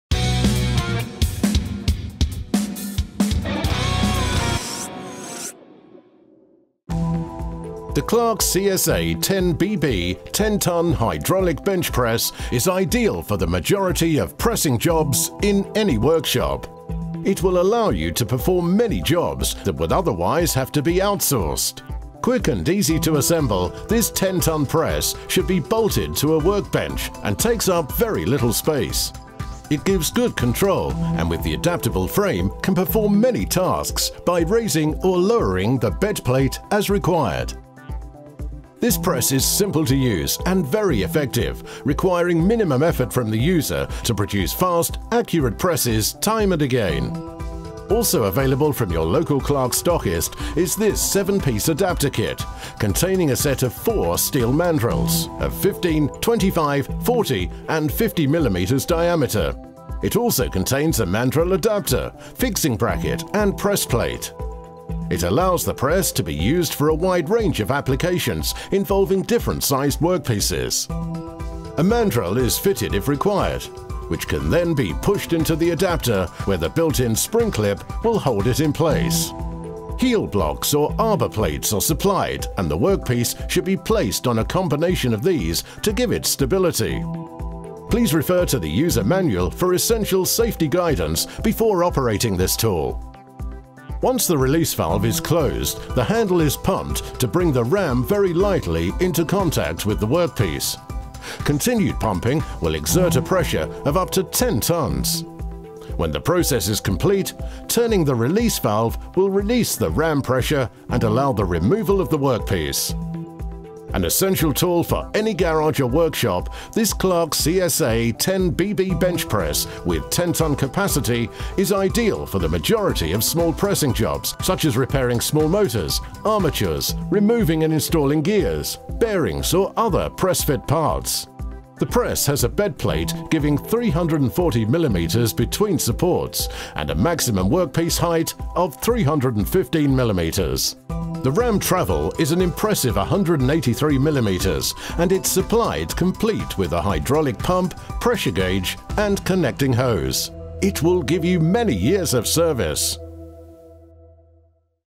Clarke CSA10BB 10 Tonne Hydraulic Bench Press - 7614020
Elevate your workshop capabilities with the Clarke CSA10BB 10 Tonne Hydraulic Bench Press. This robust bench press is essential for handling a range of small pressing tasks, including repairs and installations of gears, bearings, and other press-fit components. With a 10-tonne capacity, it delivers powerful performance with minimal effort.

Key Features:
- Powerful Capacity: 10-tonne pressing force to handle most small to medium pressing jobs with ease.
- Versatile Operation: Ideal for repairing small motors, armatures, and removing/installing gears and bearings.
- Effortless Use: Equipped with a hydraulic pump, ram with a pressure gauge, and connecting hose for straightforward operation and accurate pressing.
- Adjustable Reach: Maximum ram travel of 183mm and distance between main vertical supports of 340mm accommodate a variety of pressing needs.
- Solid Construction: Designed for durability with high-quality materials for reliable performance over time.

Specifications:
- Capacity: 10 Tonne
- Distance Between Main Vertical Supports: 340mm
-Maximum Distance of Ram to Bedplate: 315mm
- Maximum Ram Travel: 183mm
- Included: Hydraulic pump, ram with pressure gauge, and connecting hose
- Dimensions (LxWxH): 600 x 450 x 1070mm
- Weight: 51kg

Please Note: This press is supplied without the 7 Piece Adaptor Kit (Order Code: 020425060), which is sold separately.